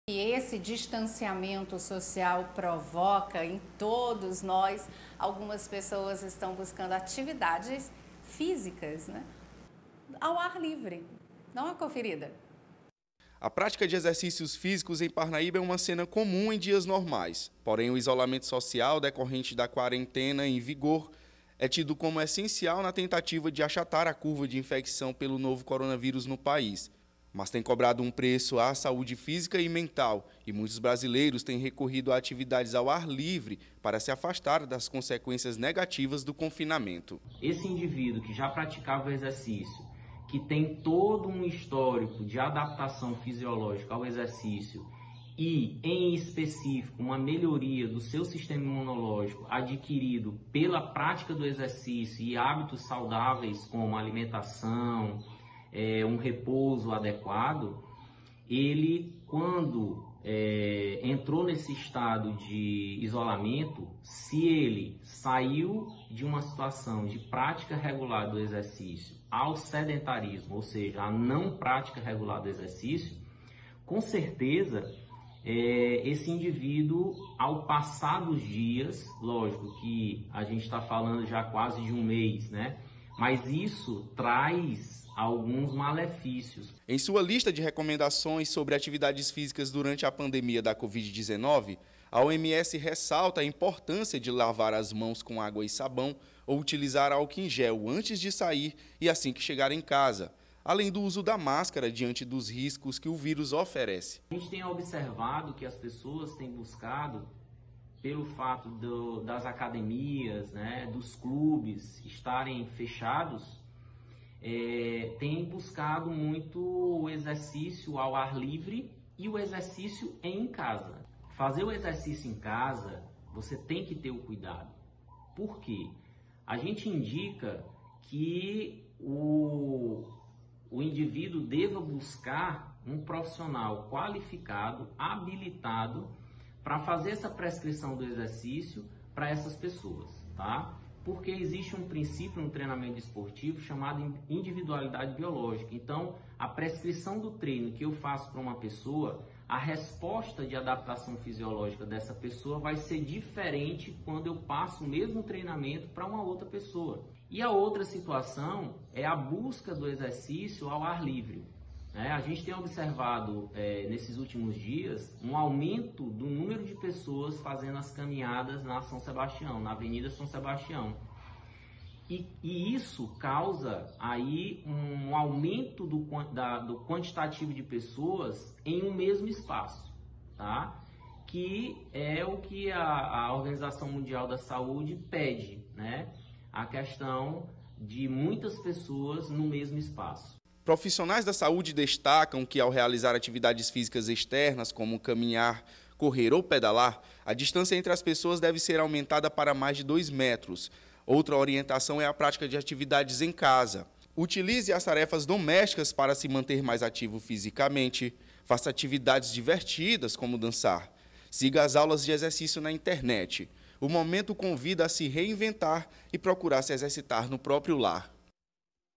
Profissional da saúde alerta consequências de atividades físicas durante isolamento social
O isolamento social devido ao contágio pelo novo coronavírus no país tem cobrado um preço a saúde física e mental de muitos brasileiros e profissionais de saúde faz alerta.